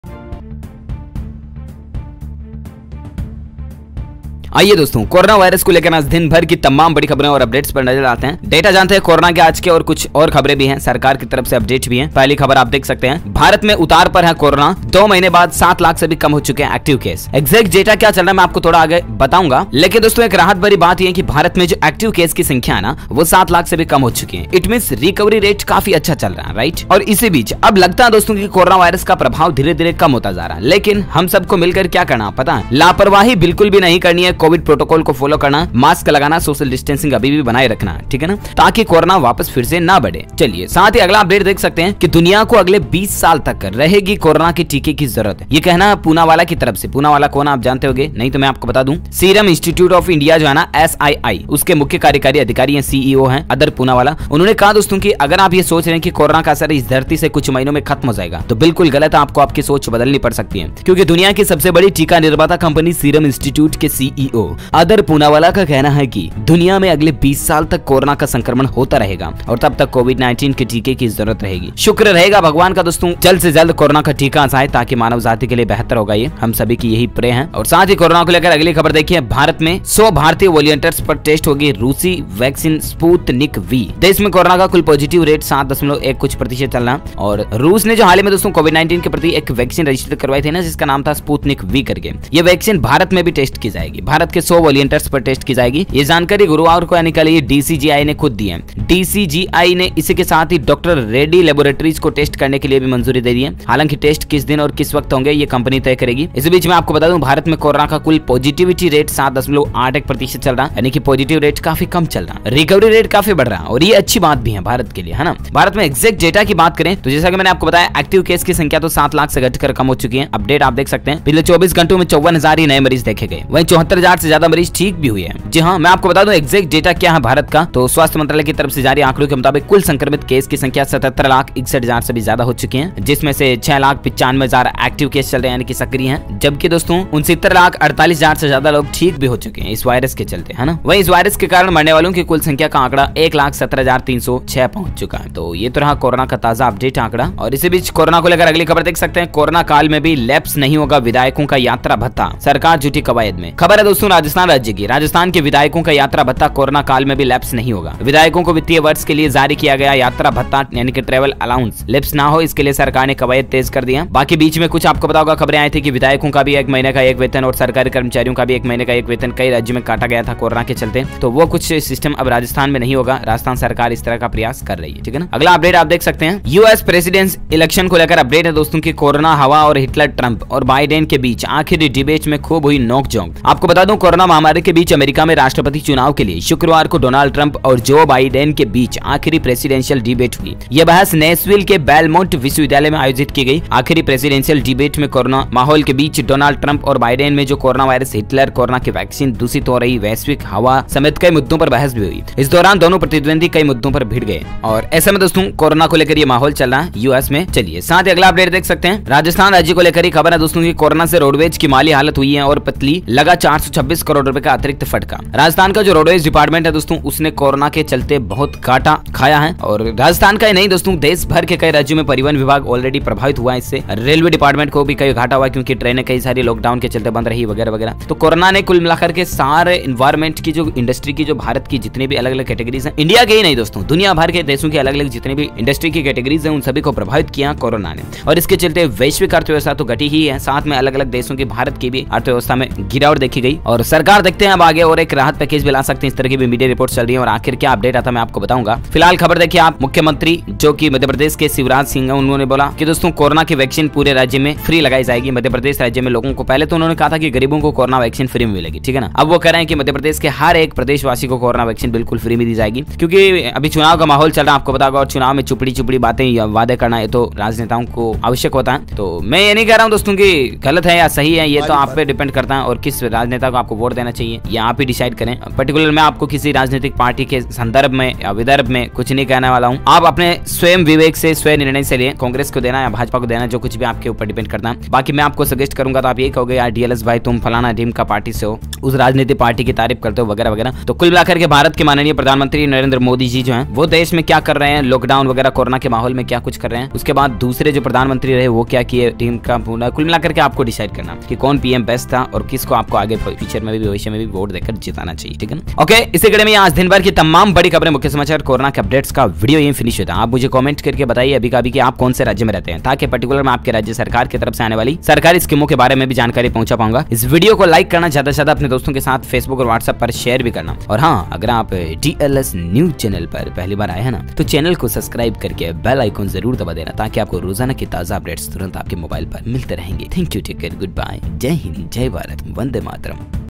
कोरोना की आज की 10 बड़ी ख़बरें - लॉकडाउन, वायरस PM Modi breaking news dls news 23 october news
DLS News Today Breaking News | DLS News aaj ki taja khabar
1. Corona Active Cases India: उतार पर कोरोना, दो महीने बाद 7 लाख से कम हुए ऐक्टिव केस
2. दुनिया को अगले 20 साल तक रहेगी कोरोना के टीके की जरूरत: पूनावाला - Corona COVID 19 World will need vaccines for 20 years says Adar Poonawalla tutd - AajTak
3. Coronavirus India Cases, Vaccine News Live Updates in Hindi: Corona Cases in India Latest News, Covid-19 Tracker Today | Bihar, Punjab, Delhi, Rajasthan - Coronavirus India LIVE Updates: 100 भारतीय वॉलंटियर्स पर टेस्ट होगी रूसी 'Sputnik V', देश में कोरोना का कुल पॉजिटिविटी रेट 7.81% - Jansatta
4. India Coronavirus Cases And Deaths Updates 23 October 2020 | भारत में कोरोना एक्टिव केस घटकर 7 लाख से भी कम हुए, 24 घंटे में आए 54 हजार मरीज, 74 हजार ठीक हुए
5. Rajasthan: कोरोना काल में भी लैप्स नहीं होगा विधायकों का यात्रा भत्ता, सरकार जुटी कवायद में
6. US Presidential Election 2020: कोरोना, हवा और हिटलर-ट्रंप और बाइडेन के बीच आखिरी डिबेट में खूब हुई नोकझोंक
7. Rajasthan: कोरोना से रोडवेज की माली हालत हुई और पतली, लगा 426 करोड़ रुपये का अतिरिक्त फटका
8. शिवराज बोले, मध्य प्रदेश में गरीबों को कोरोना की वैक्सीन मुफ्त - Regional News - Samay Live
9. Video details - YouTube Studio